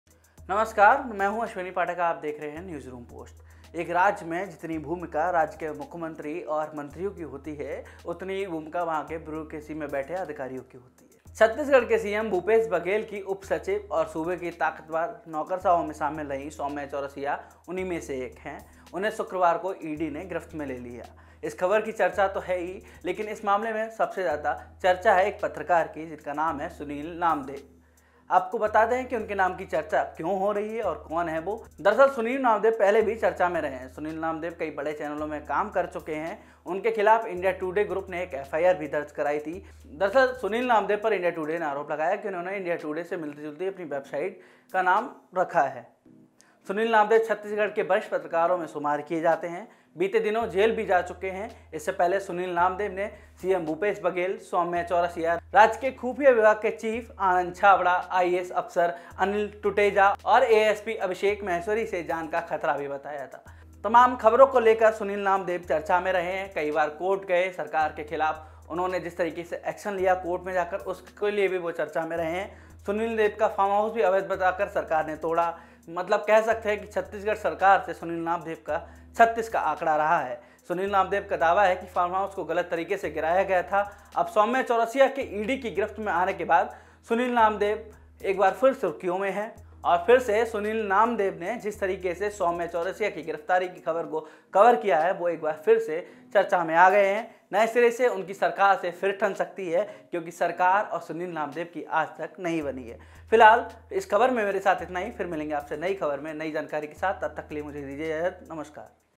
Chhattisgarh:कौन हैं पत्रकार सुनील नामदेव जिनकी ‘भ्रष्टाचारी’ IAS अफसर पर रिर्पोटिंग की हो रही चर्चा
ChhattisgarhIकौन हैं पत्रकार सुनील नामदेव जिनकी ‘भ्रष्टाचारी’ IAS अफसर पर रिर्पोटिंग की हो रही चर्चा

Chhattisgarh News
Sunil Namdeo